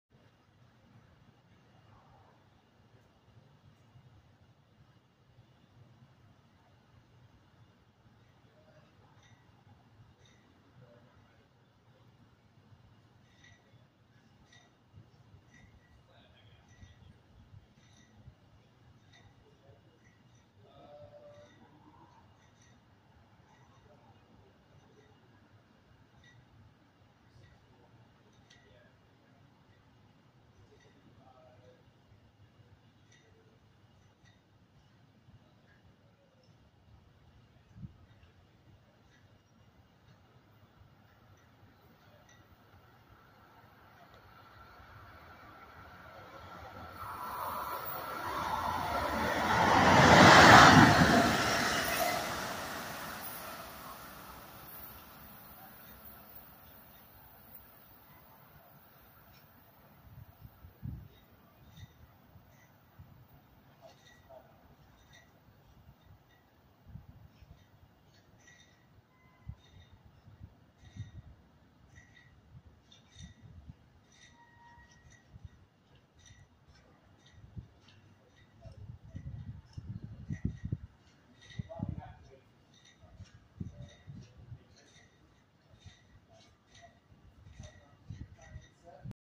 NJ Transit Equipment Move at Hamilton
An NJ Transit non revenue equipment move to Morrisville Yard with ALP-46 4626 pulling another locomotive & a bi-level coach streaks west through the station at Hamilton around 9:10 PM before I called it a night. Had I not came here I would've missed out completely.